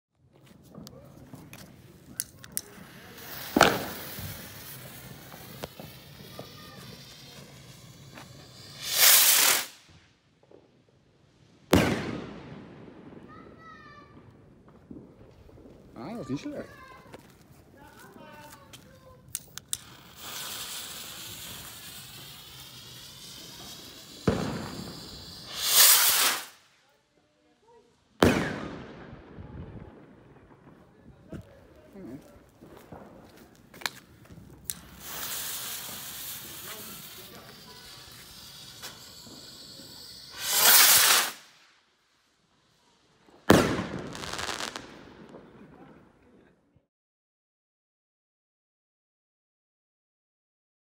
Original Fireworks Pro Rockets One - Oud & Nieuw 2018-2019 - Pyromaniac Vuurwerk Video's
Pro Rockets One, 3 pijlen van de Original Fireworks: Pro Rockets One pakket.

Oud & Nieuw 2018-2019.

© Pyromaniac Vuurwerk Video's